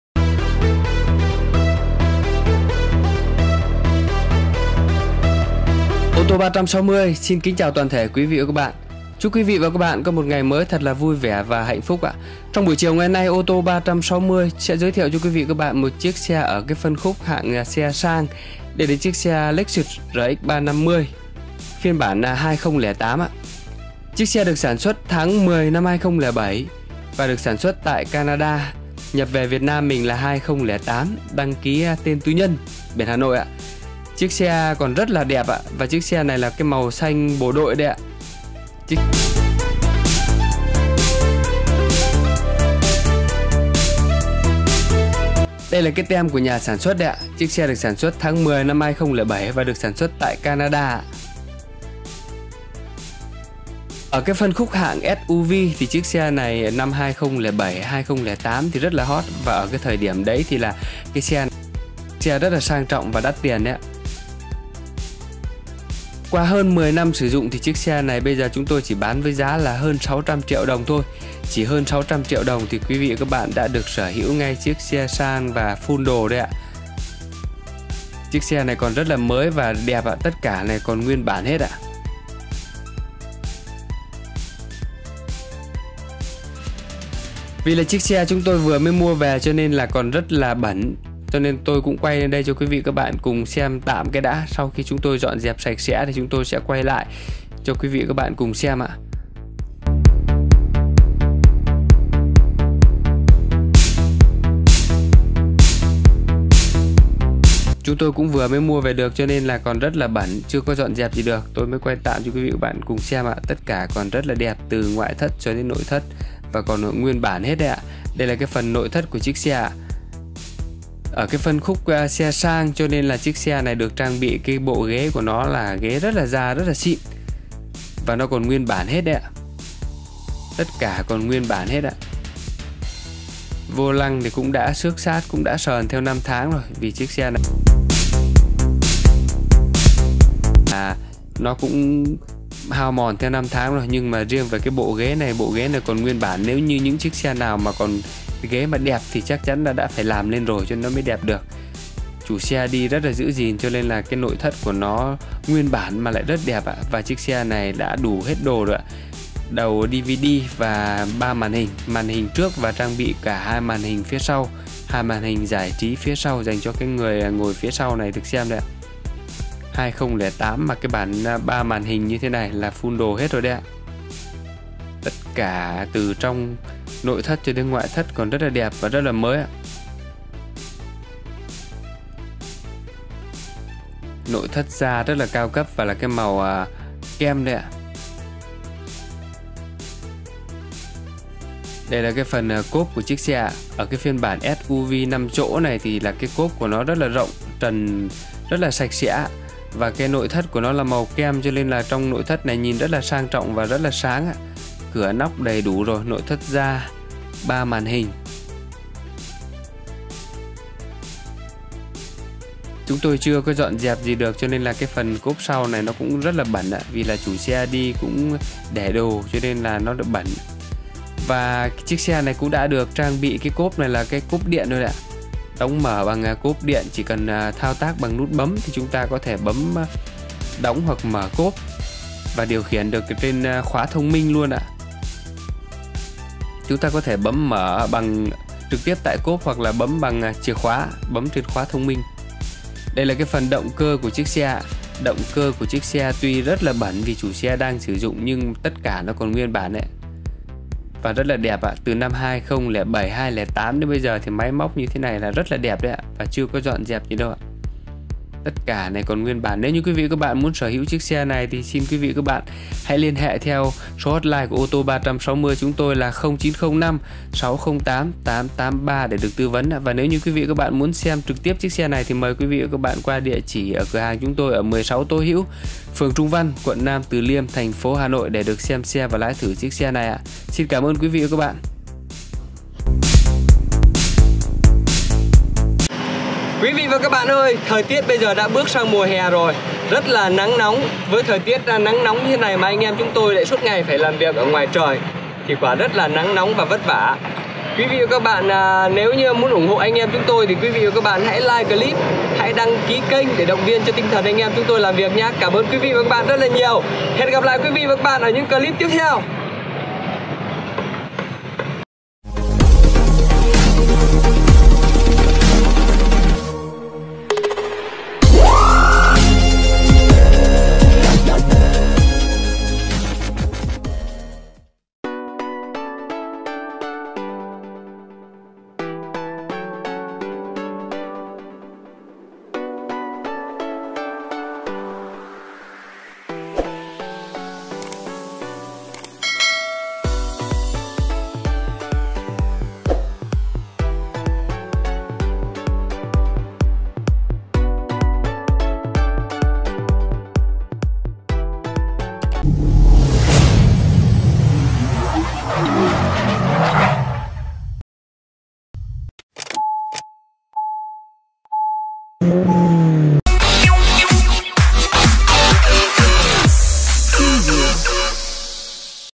Hơn 600tr mua được ngay xe sang nhập Mỹ Lexus RX 350 2008 rất sang chảnh
Bán xe Lexus Rx 350 nhập khẩu Mỹ sản xuất tháng 10/2007 đăng ký 2008 đăng ký tên tư nhân, biển Hà Nội. Màu xanh bộ đội.
Xe được trang bị động cơ xăng 3.5L hộp số tự động, nội thất da cao cấp, ghế chỉnh điện, đầu DVD 3 màn hình giải trí, âm thanh cao cấp, cửa nóc, rửa đèn pha, cốp điện, xe còn rất mới, máy móc nguyên bản, nội thất đẹp, khung gầm chắc chắn.